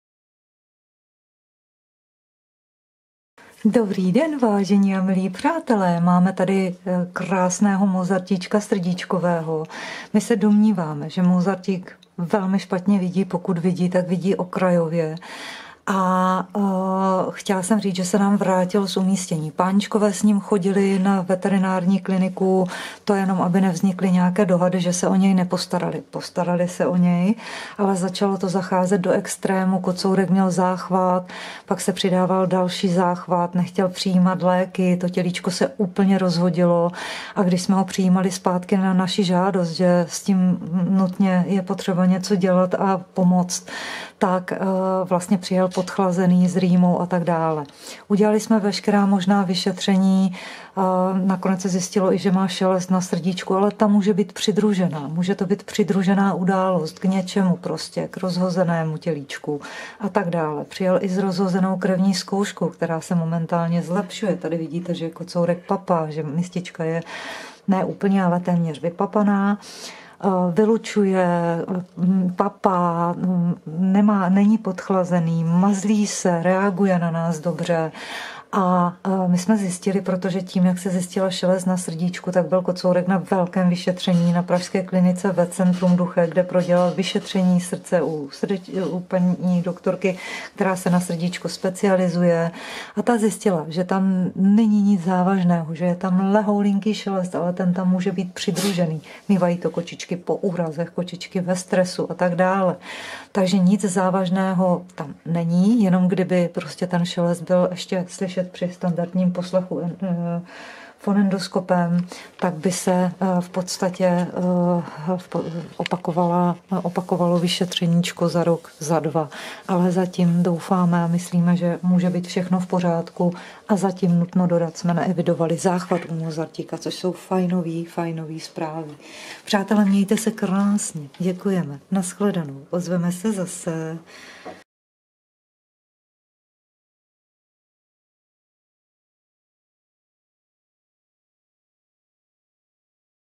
Mozartíček (9.3.2022)
Krásný chlapík Mozartíček níže kopíruje nedávný veterinární text a dnes přidává video pozdrav z hospitalizačního boxíčku. Mrkněte na video, dozvíte se více. Kocourek má za sebou vyšetření srdíčka. Držíme tlapičky, přejeme co nejvíce zdravíčka a jedeme dál.
"Nádherný Mozartíček má za sebou vyšetření srdíčka na pražské klinice Vetcentrum Duchek Vyšetření dopadlo uspokojivě. Na srdíčku je slabá šelest, ale ta může být způsobená stresem, manipulací či nějakou přidruženou událostí. Žádná závažná srdeční vada nebyla odhalena. Zveřejňujeme fotografie ještě z vyšetření v útulku pod záštitou paní doktorky Veroniky Kadlecové, která si kocourka poslechla a šelest na srdíčku odhalila. Milovaný Mozartíček se vrátil do útulku z nového domova, kde začal mít záchvaty a jedno se přidružovalo k druhému. V útulku zjištěná rozhozená krevní zkouška se nyní zlepšuje, záchvat zatím evidován nebyl, kocourek papá, mazlí se a to jsou zatím příznivé zprávy. Ve prospěch nemocného kocourka Mozartíka tímto vyhlašujeme veřejnou sbírku: poznámka "Mozartík" v.s. 82022 na účet: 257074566/0300 anebo na transparentní účet: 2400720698/2010. Dary útulku lze poskytovat také prostřednictvím portálu Darujme.cz Velmi děkujeme."